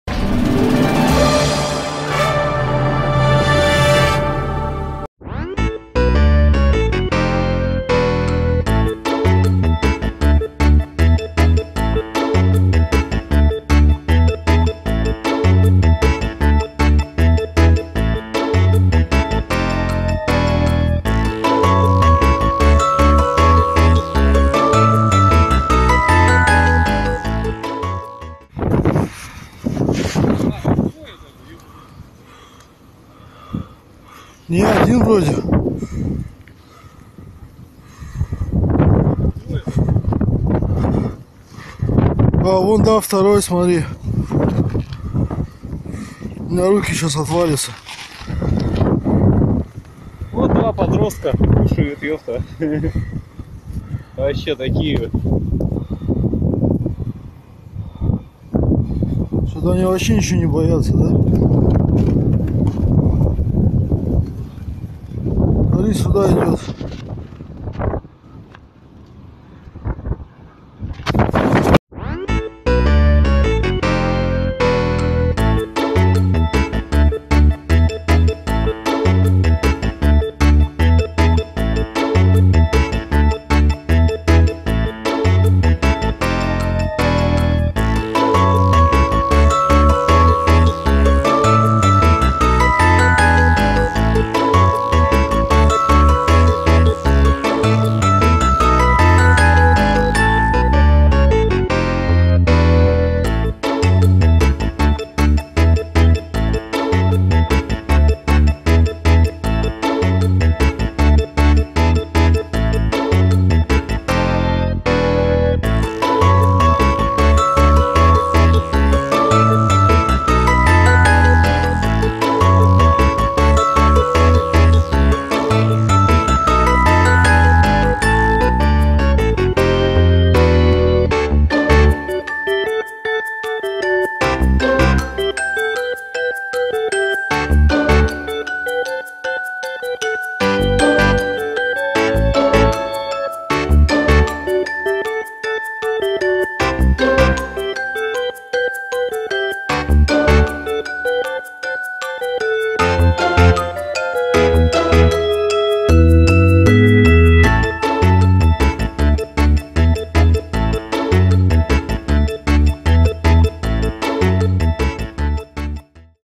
На Новой Земле массовое нашествие белых медведей
На Новой Земле введен режим чрезвычайной ситуации из-за массового нашествия белых медведей.  Об этом сообщает пресс-служба правительства Архангельской области. В
районе поселка Белушья Губа находятся 52 медведя. Они ведут себя агрессивно, нападают на людей и проникают в помещения. Пострадавших пока нет, но люди напуганы,
боятся выходить из дома и отпускать детей в школу. Обычные меры безопасности не действуют: медведи не реагируют на свето-шумовые сигналы и на попытки отогнать их при помощи патрульных автомобилей и собак. Нашествие белых медведей в поселках архипелага Новая Земля связывают с сезонной миграцией животных. Медведи идут с южной части архипелага, где лед начинает подтаивать, на север, где лед еще устойчивый. В поселках они останавливаются из-за свалок с пищевыми отходами. Если бы их не было, то животные прошли бы мимо. Ученые отмечают, что такое количество белых медведей в этом районе ранее не фиксировалось.